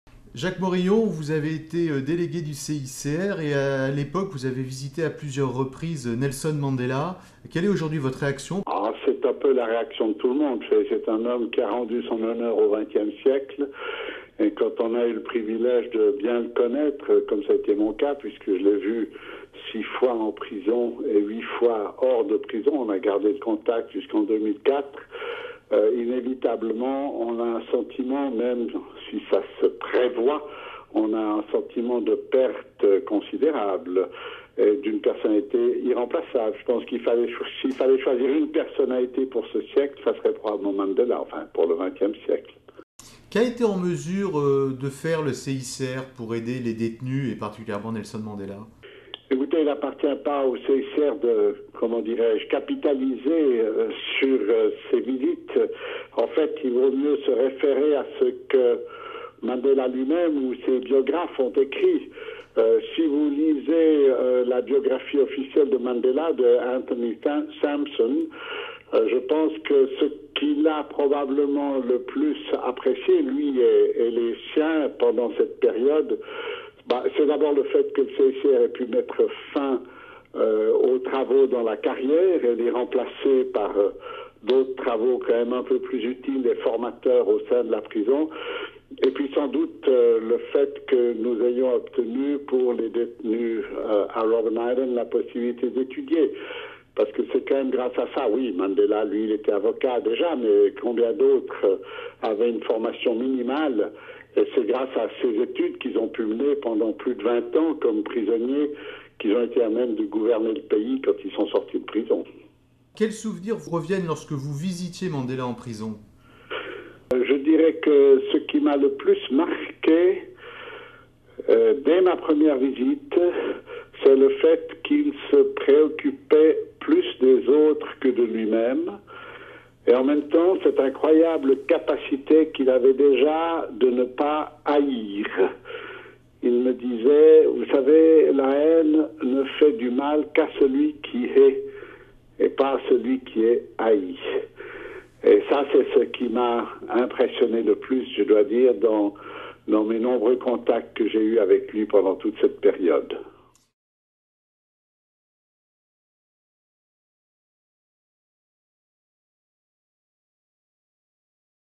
Nelson Mandela : souvenir d'un délégué du CICR
Jacques Moreillon, ancien vice président du CICR, a, dans les années 70, alors délégué de l'Institution, visité en détention, plusieurs fois, Nelson Mandela. Retour sur cette expérience.
Centre de production multimedia
Délégation du CICR en France
Réalisation : Lucile Marbeau - Frédéric Joli
L'humanitaire dans tous ses Etats